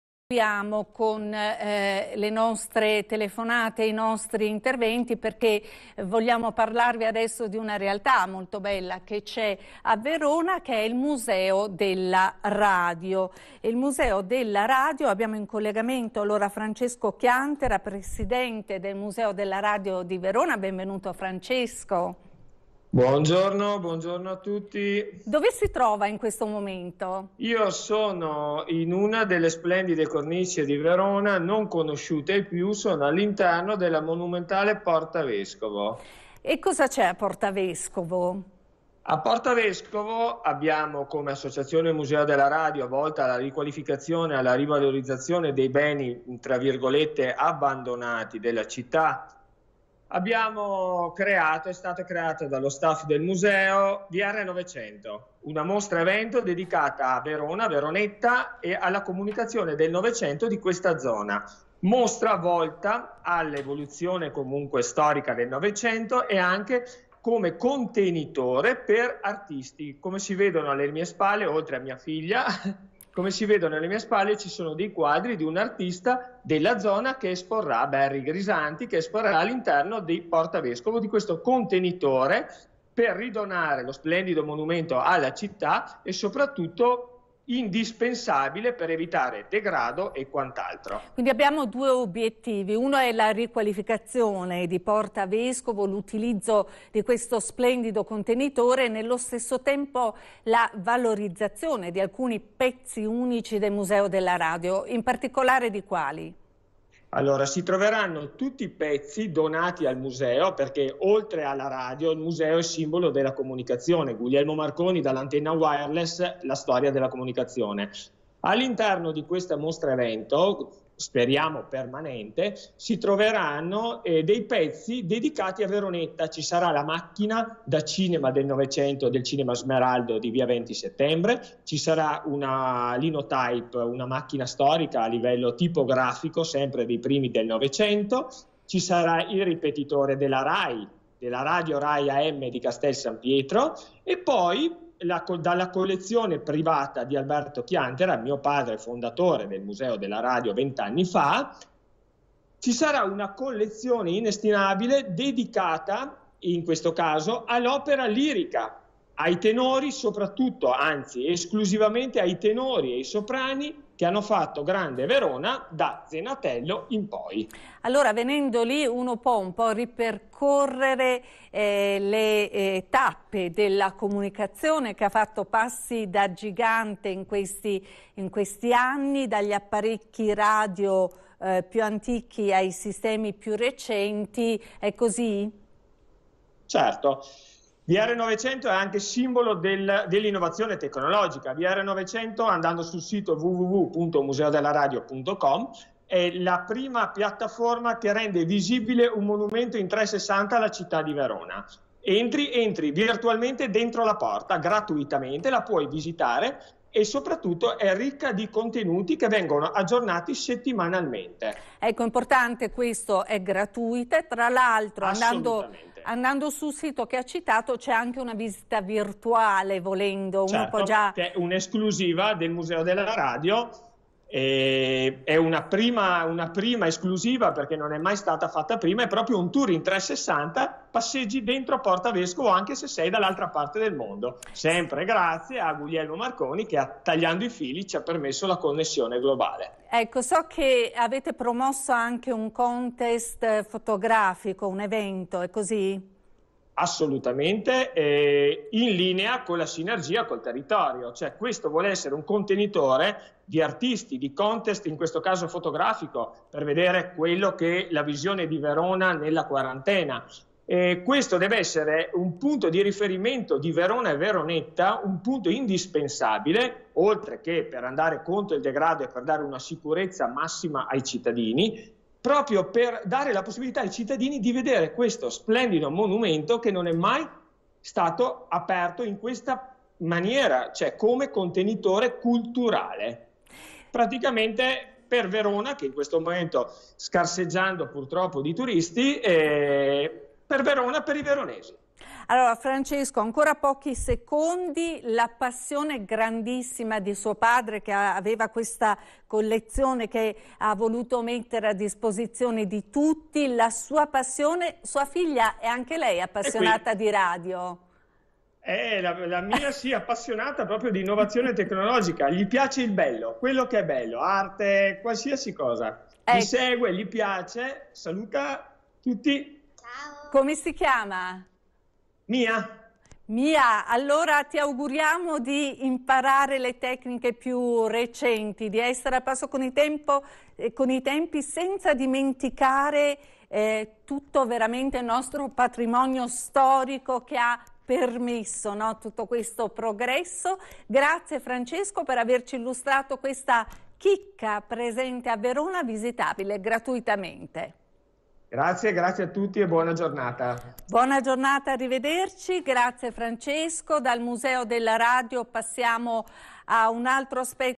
Museo della radio vr900 telepace
A Porta Vescovo sarà presentato il concept del nuovo contenitore culturale dedicato a Veronetta, vr900: un progetto volto a recuperare un patrimonio storico e restituirlo alla città, riqualificato grazie alla cultura ed aperto a sinergie con il tessuto sociale circostante, nel segno dell'innovazione tecnologica, senza dimenticare il valore del sito che lo ospita.